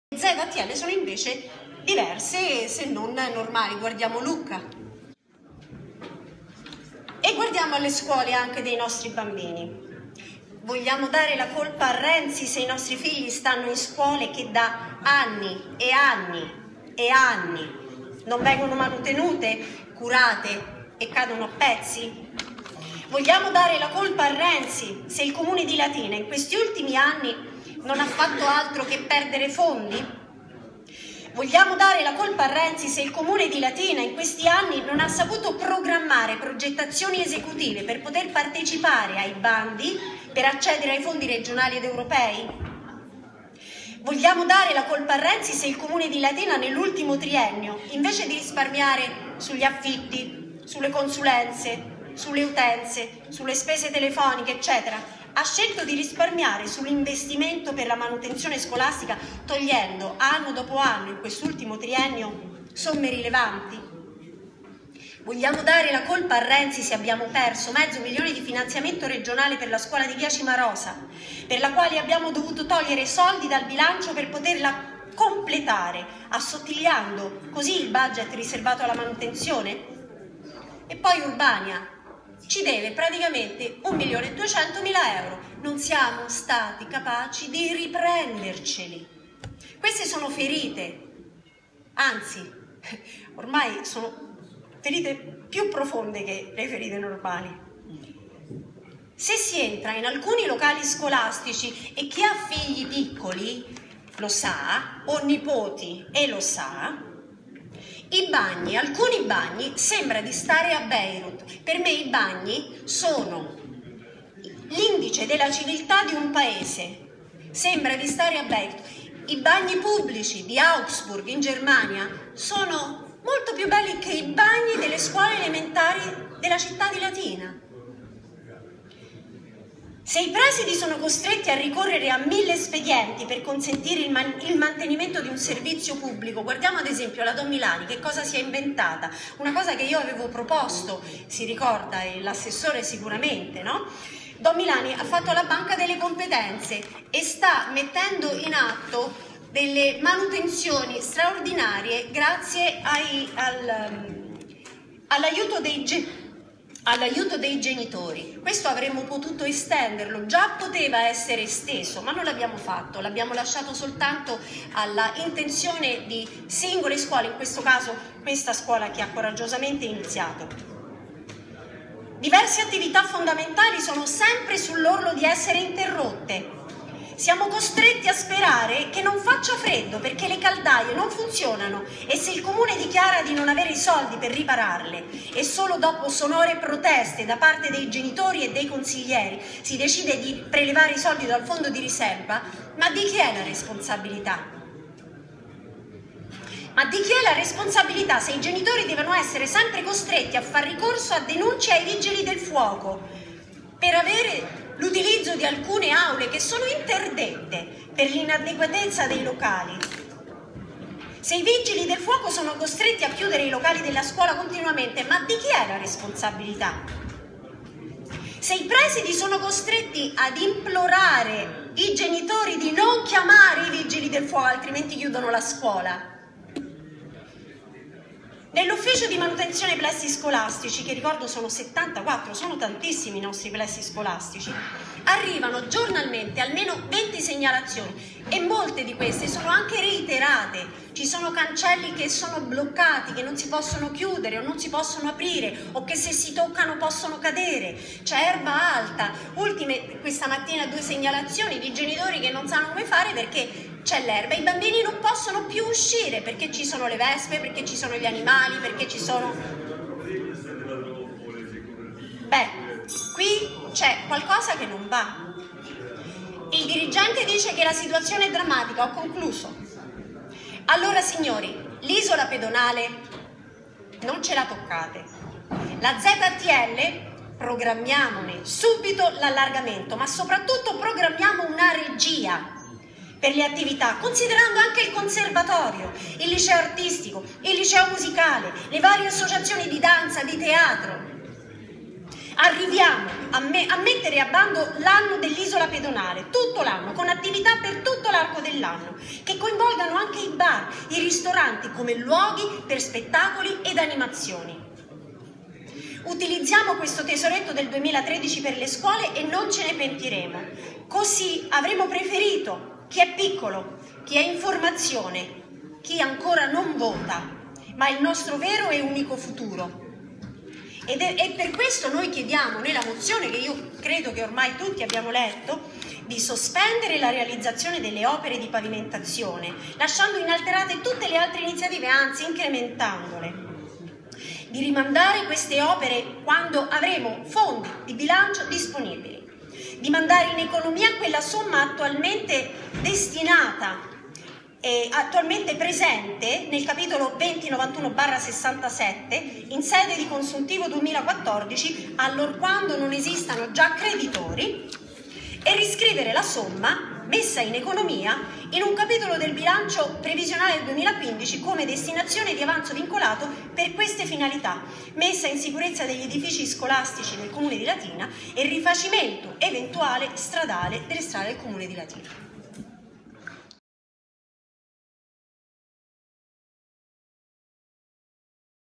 Condizioni delle scuole di Latina: di chi le responsabilità?
presentazione della mozione che prevede la sospensione della pavimentazione della ZTL a favore delle scuole del Comune di Latina. Consiglio comunale del 15 aprile 2015. Illustrazione e intervento della consigliera Nicoletta Zuliani, partito democratico. Terza ed ultima parte.